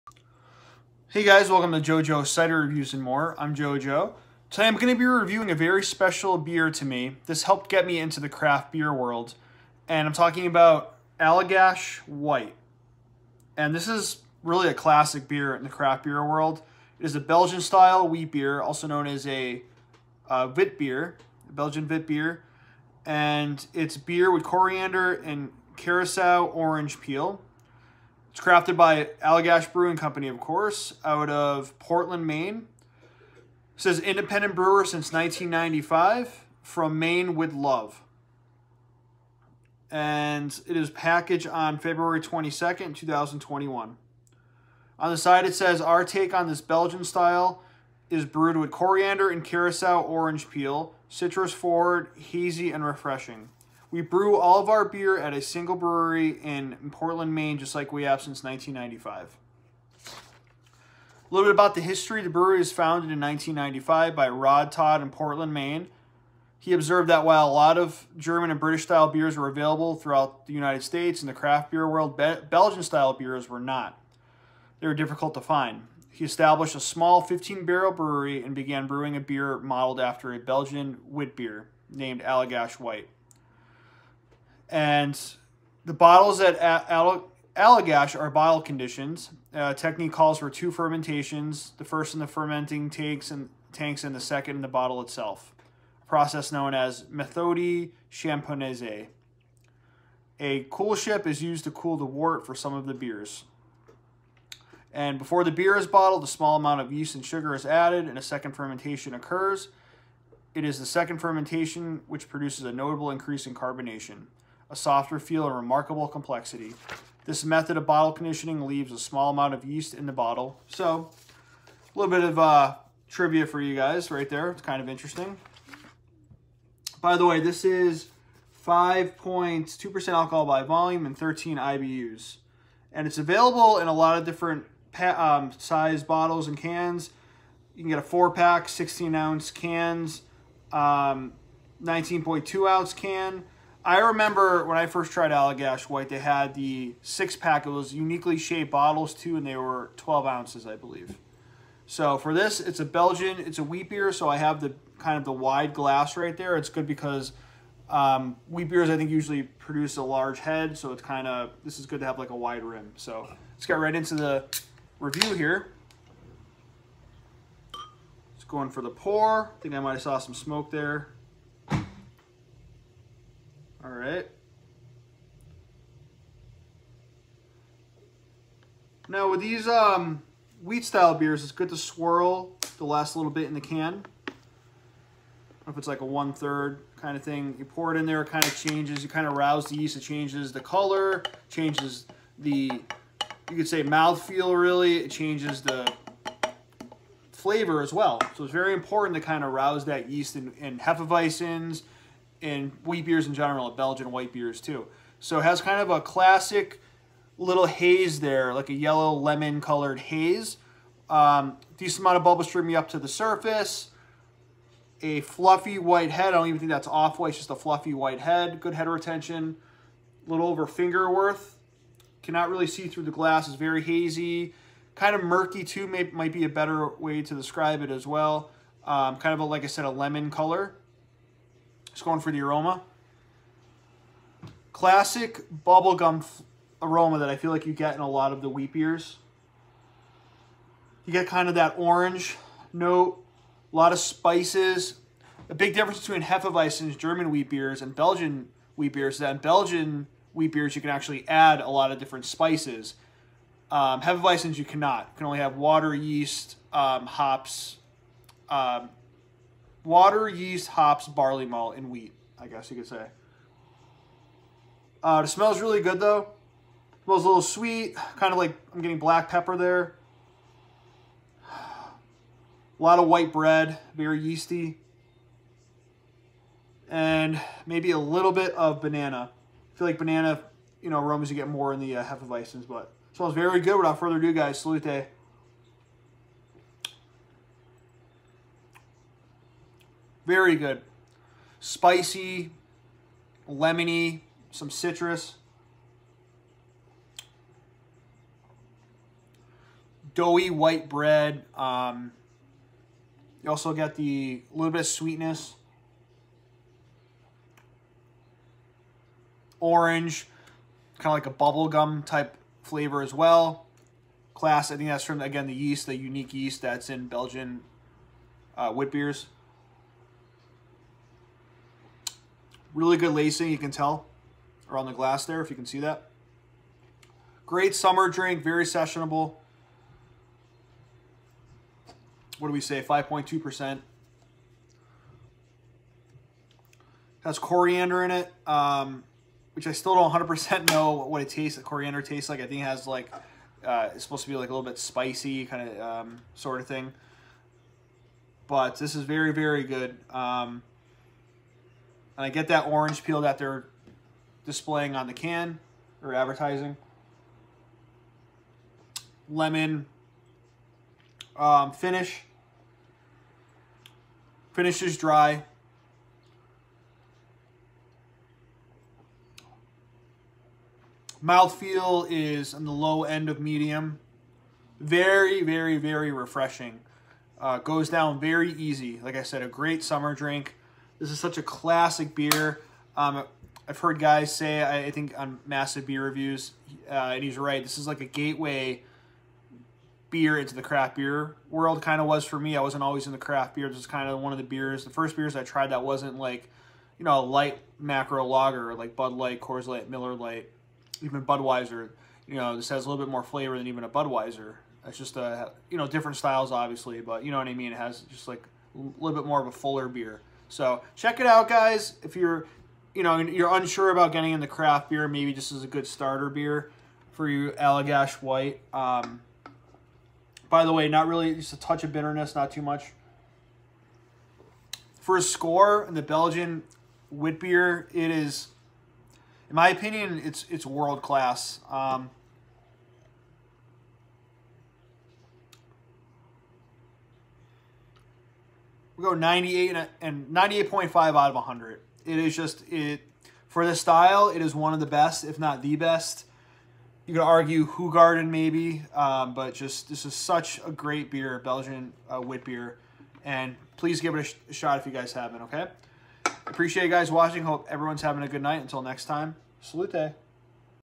Allagash White Review
A review of this classic in the craft beer world